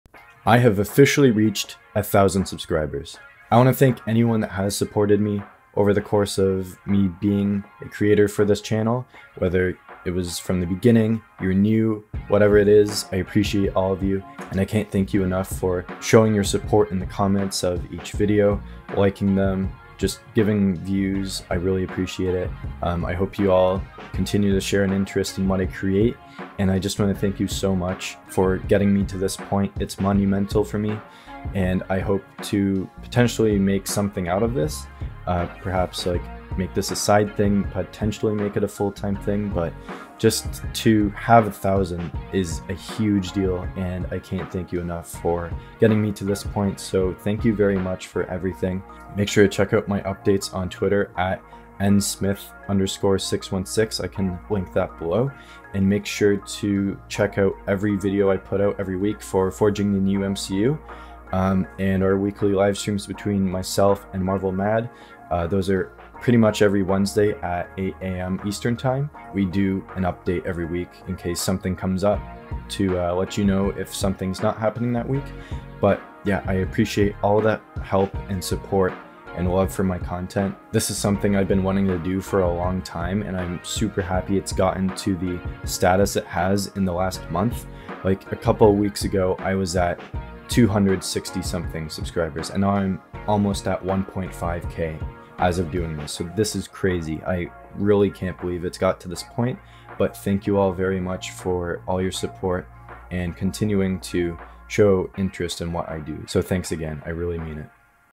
Thank You for 1000 Subscribers!
It's official!

I have finally passed 1000 subscribers after over a year of making content for this channel and brand.

Thanks to everyone who helped me reach this milestone. The grind doesn't end here...